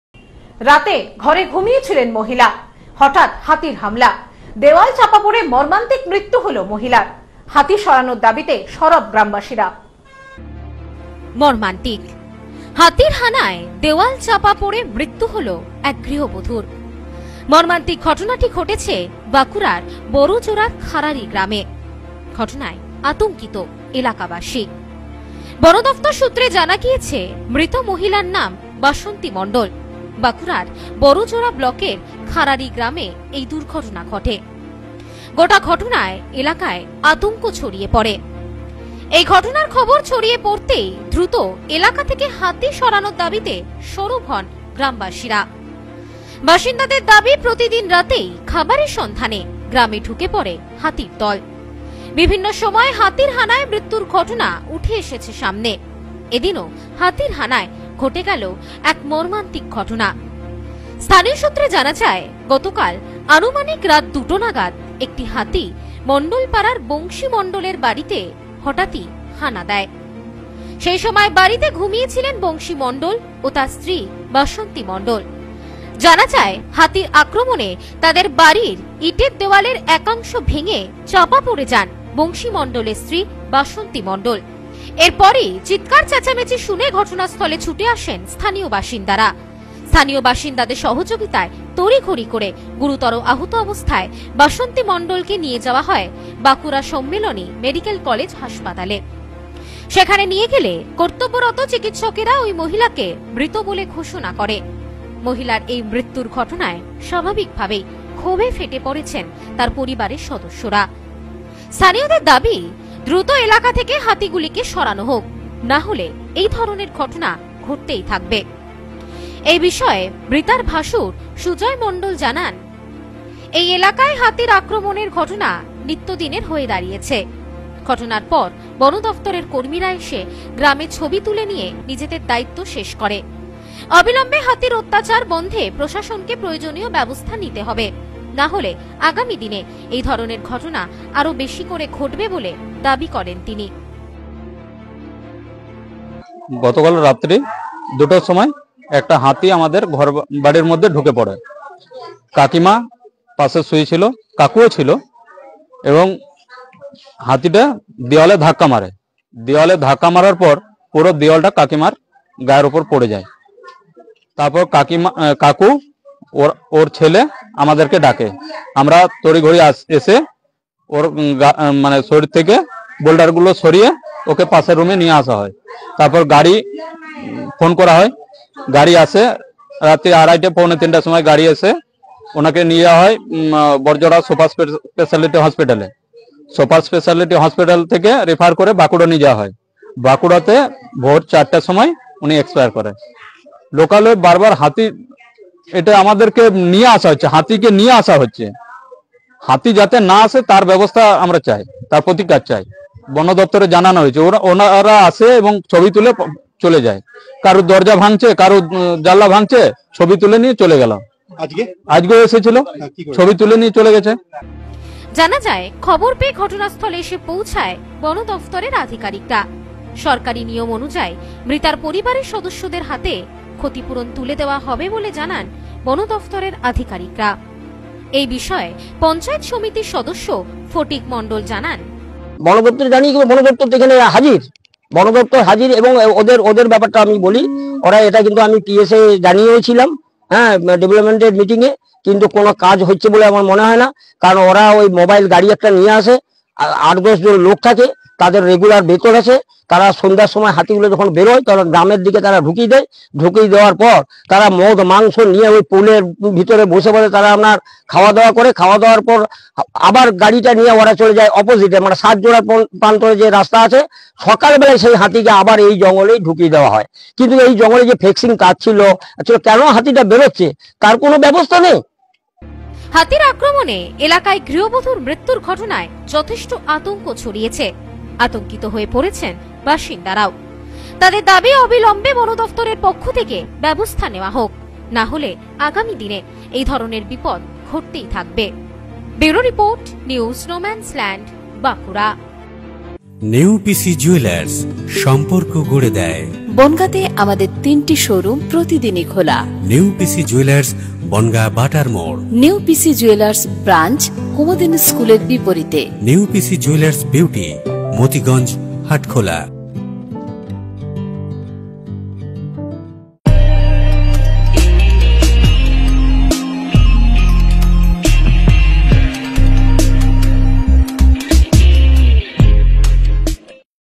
রাতে ঘরে ঘুমিয়ে ছিলেন মহিলা, হঠাৎ হাতির হামলা।
রাতে ঘরে ঘুমিয়ে ছিলেন মহিলা, হঠাৎ হাতির হামলা। দেওয়াল চাঁপা পড়ে মর্মান্তিক মৃত্যু। হাতি সরানোর দাবীতে সরব গ্রামবাসীরা।